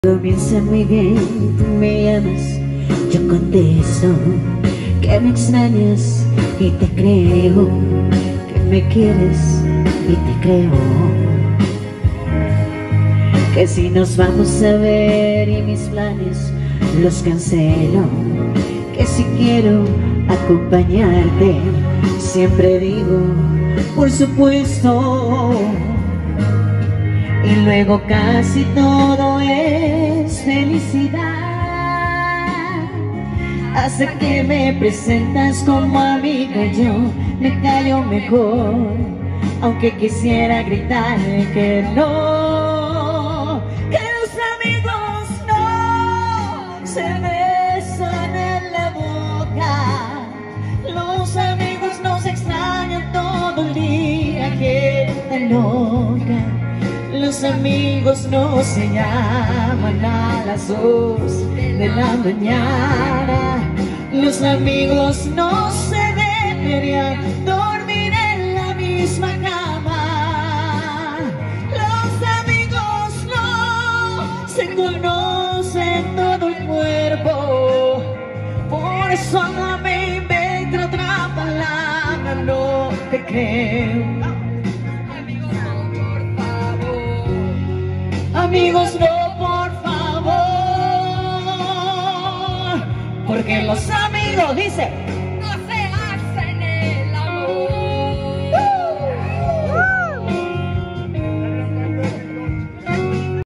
Dúo Artístico Musicales Canto de la Luna y Despecho Mode. Tel.555616-1681 CelWhats 551989-8574
Dúo Artístico Musicales ameniza kilométrica comida con una duración de 10 horas con DJ y Karaoke.